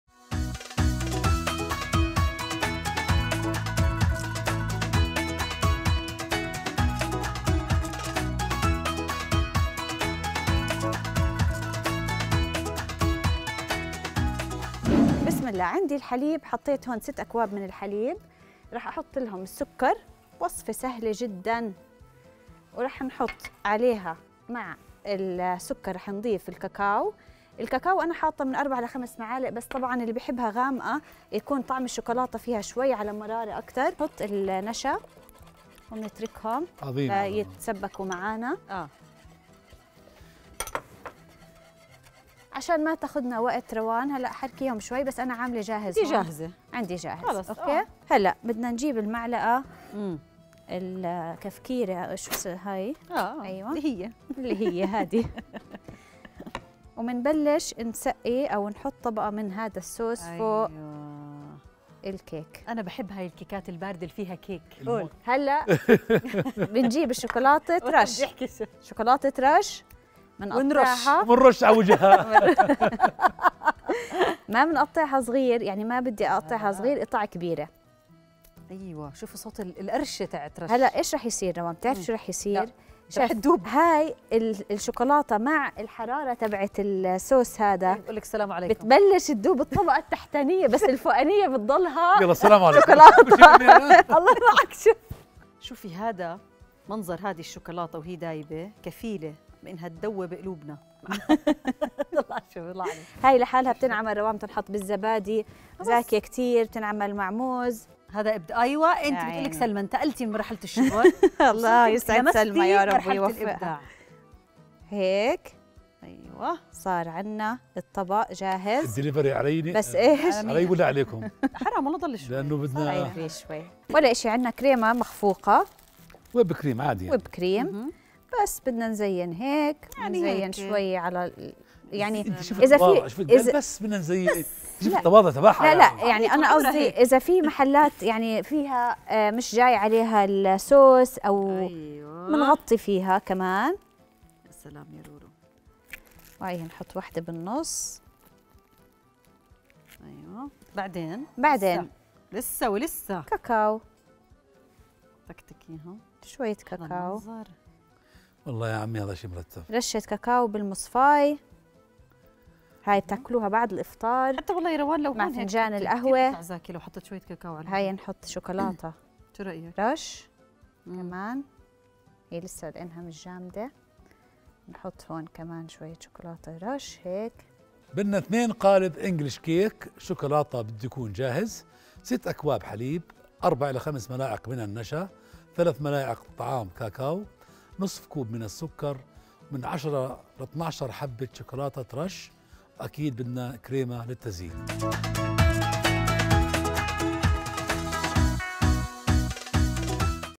حلى الراش بطريقه مميزة
حلى الشوكولاتة
المكونات :
٢ قالب انجليش كيك شوكولاته جاهز
٦ أكواب حليب
4 الي 5 ملاعق نشا
٣ ملعقة طعام كاكاو
١/٢ كوب سكر
١٠-١٢ حبة شوكولاتة رش
كريمة للتزيين

مطبخ رؤيا